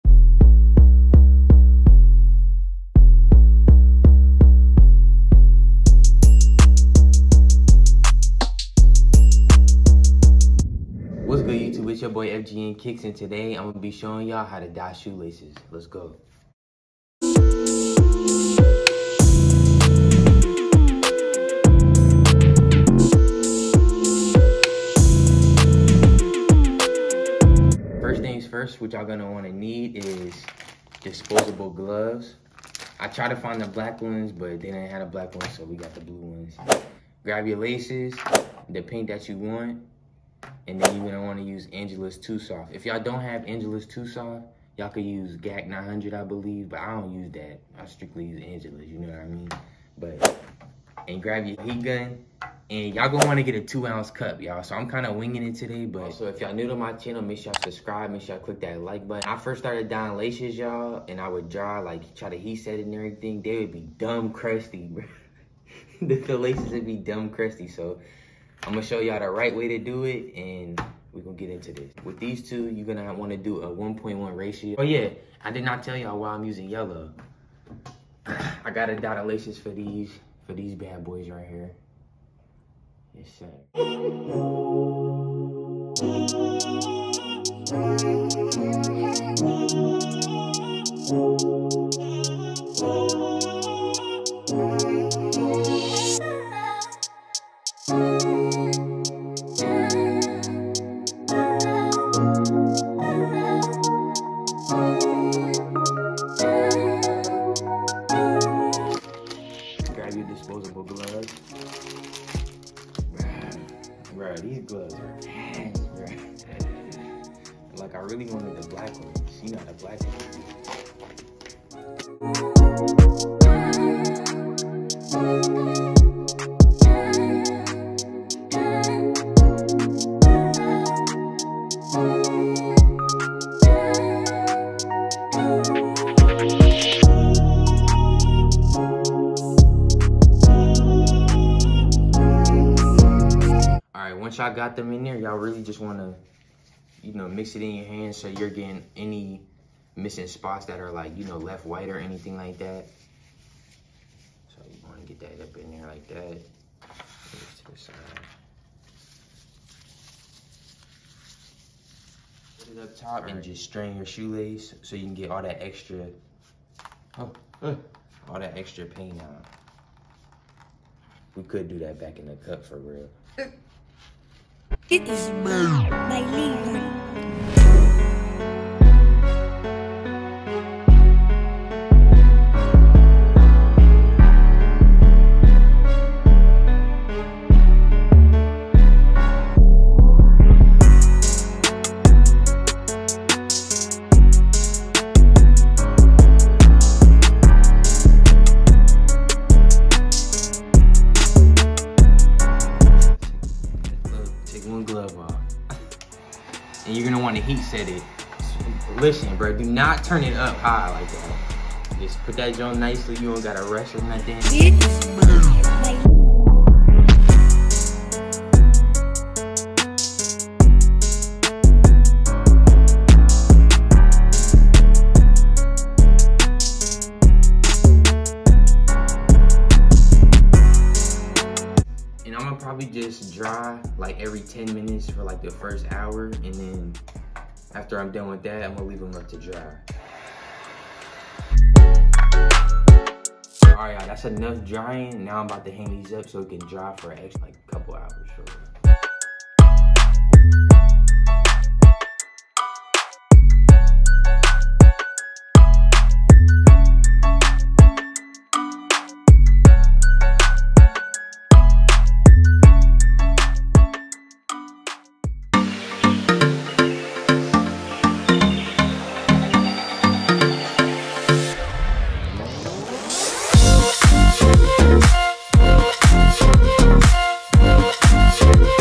HOW TO DYE SHOE LACES | EASY TUTORIAL
In this video I will show you how to dye shoe laces, in this easy tutorial you will learn how to stop dying those laces and getting those crusty results!!

OHH! & Smash that like 👍🏽 button & 🔔!!